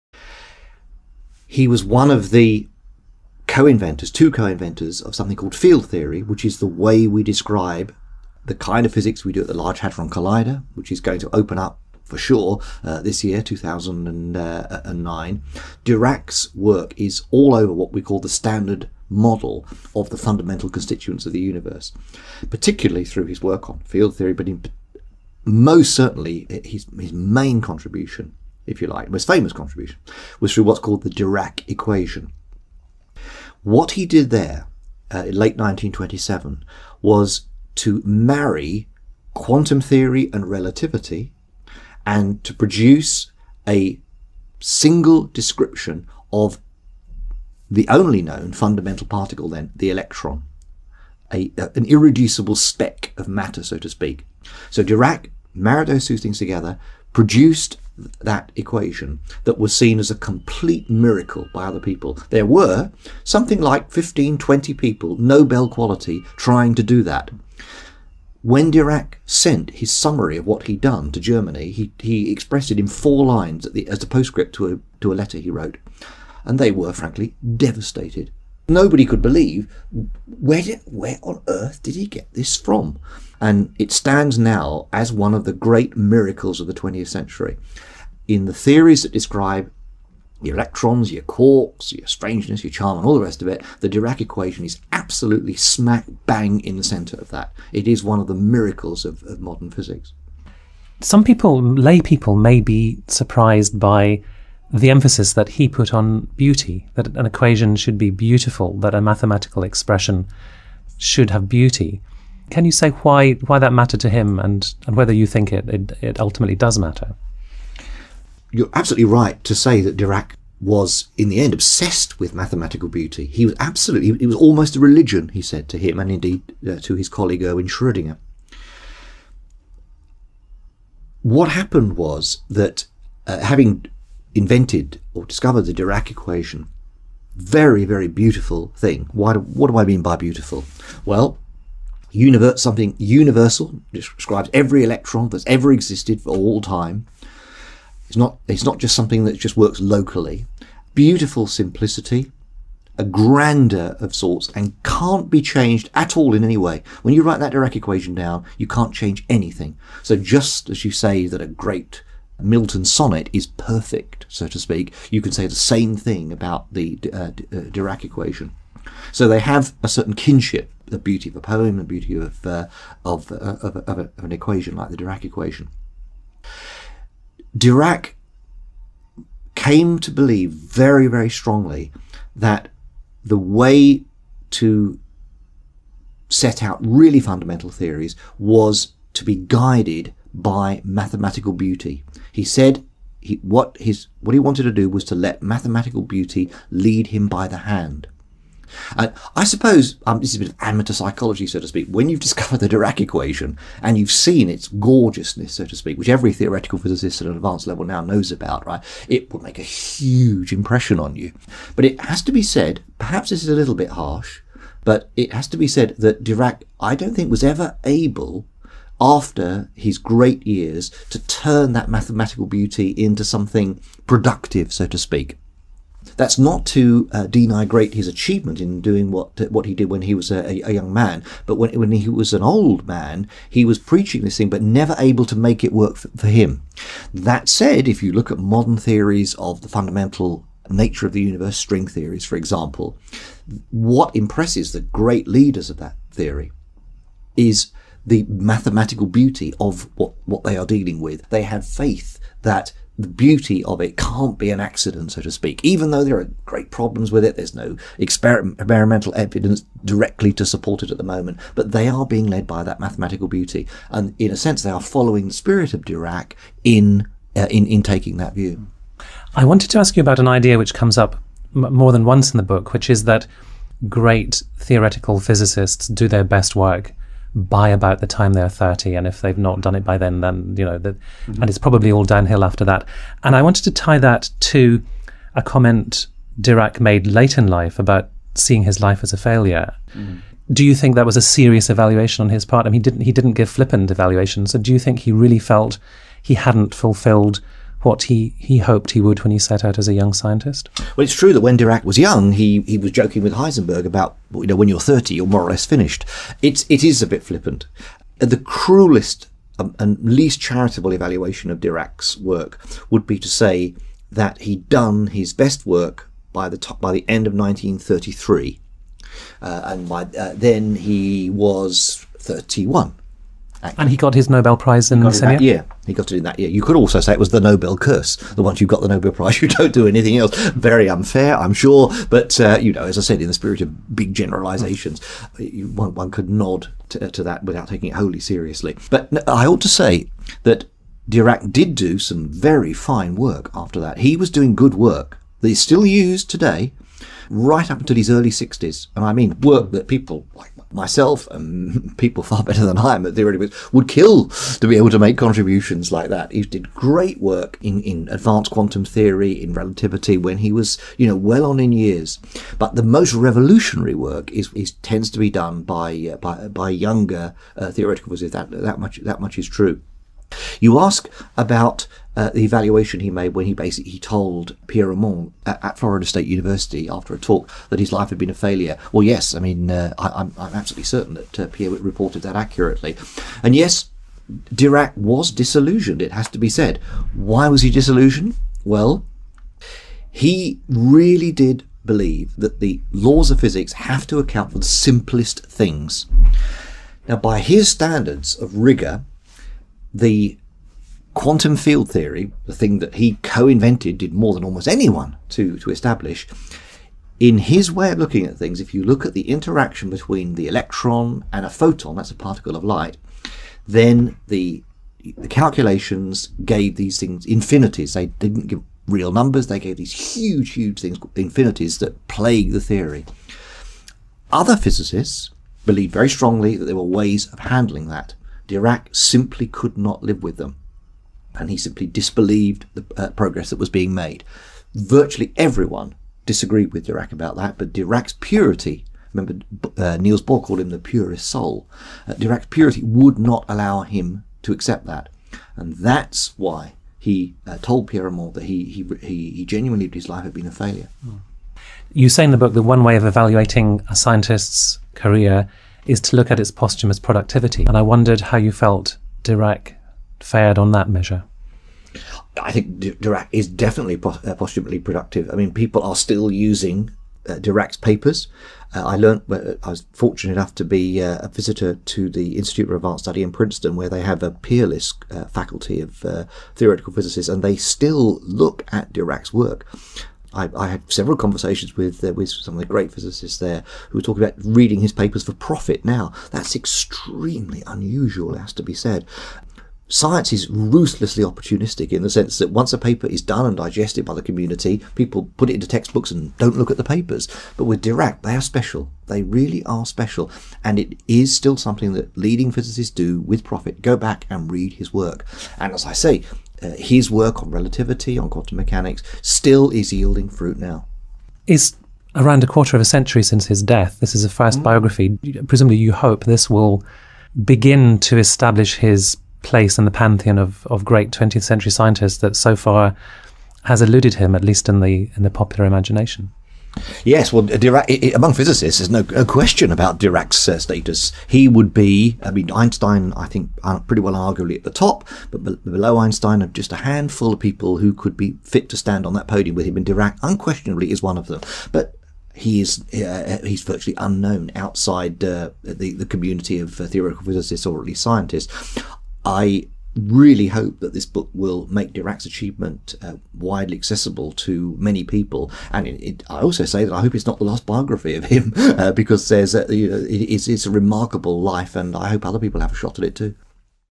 Graham Farmelo - The Strangest Man - Part 3 of 3
Graham Farmelo's 'The Strangest Man' looks at the extraordinary life of the theoretical physicist, Paul Dirac. Little-known beyond the scientific community, Dirac was one of the leading pioneers in quantum mechanics, arguably the greatest scientific discovery of all time. Yet outside of his professional work Dirac was a curious character: pathologically reticent, strangely literal-minded and legendarily unable to communicate or empathize. With 'The Strangest Man' shortlisted for this year's Costa Book Awards, our regular podcast host George Miller spent some time with Graham Farmelo to discuss the quantum genius.